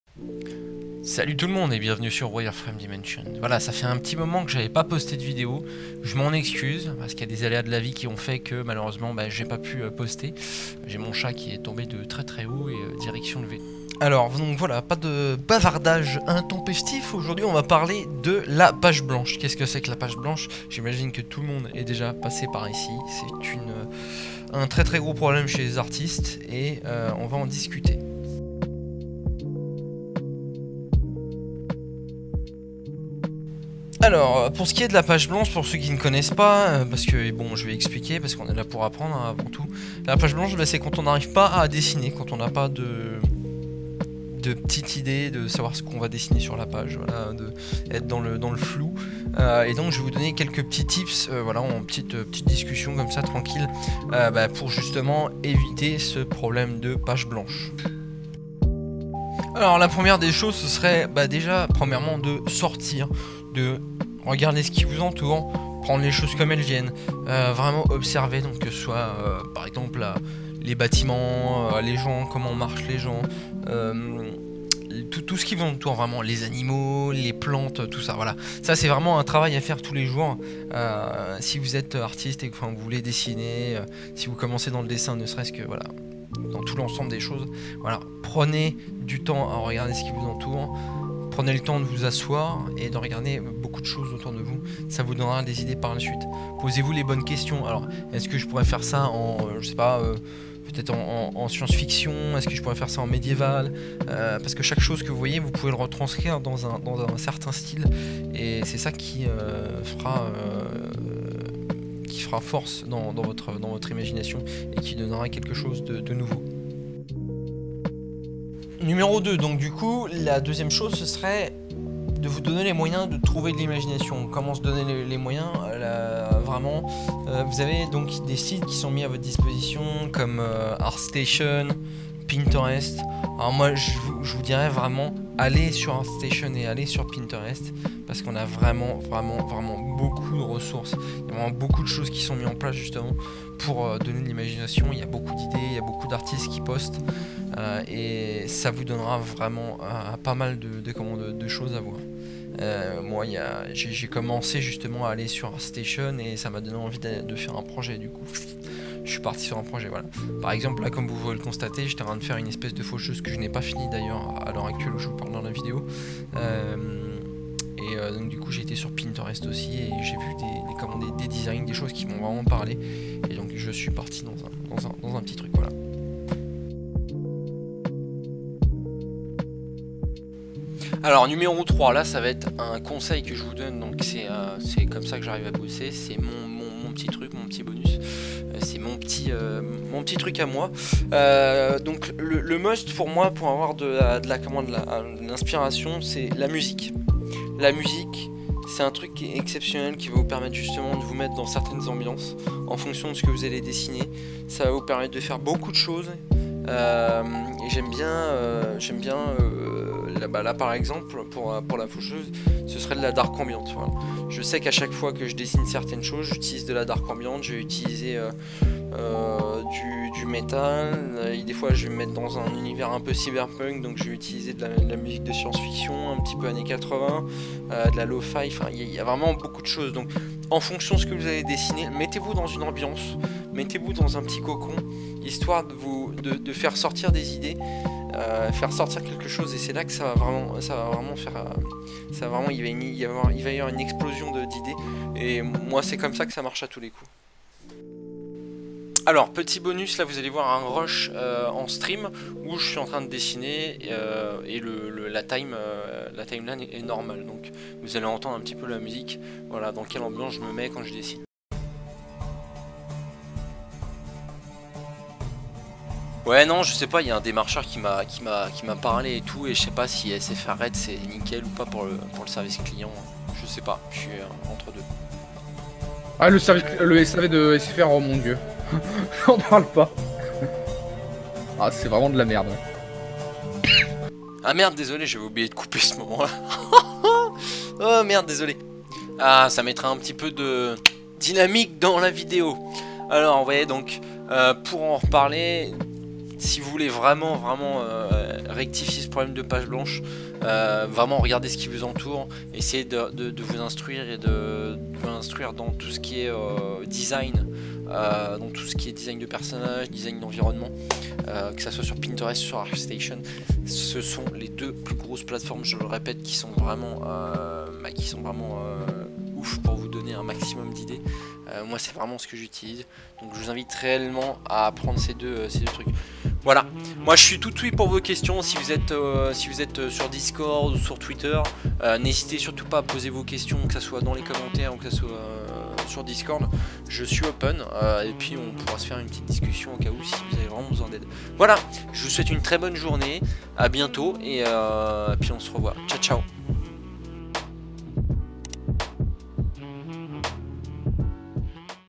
comment éviter la page blanche
Une petite vidéo discussion  sur le gros problème de la page blanche !!

N'hésitez pas a mettre des commentaires si toute fois il y'a quelque chose qui vous échappe, venez sur discord si vous avez besoin d'un coup de main pour une retouche ou des conseils !! a bientôt !!
Catégorie